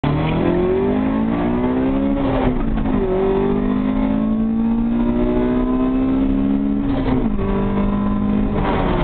B16 CRX vs 5.0 mustang 0-75mph
B16 crx with simple bolt-ons and LS transmission. 5.0 Mustang FULL bolt-on